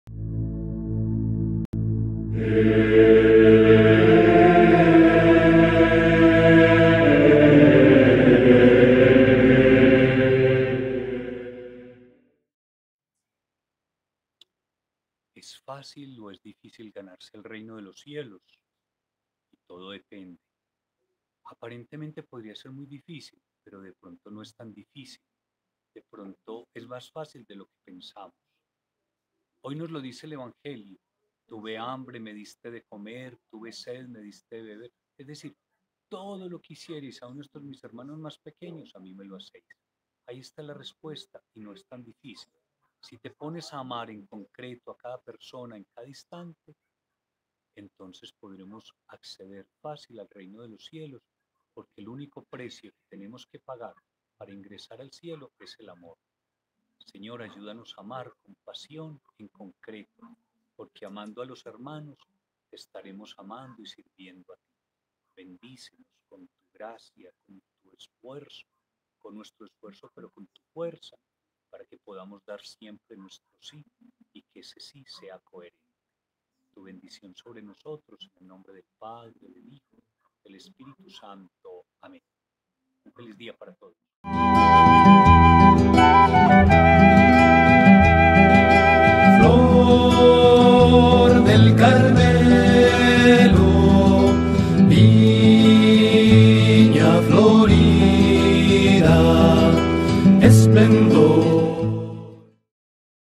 Un Mensaje de Esperanza y Vida 🙏🙏
Fr. José Arcesio Escobar Escobar Desde la Ciudad de Dios de Villa de Leyva.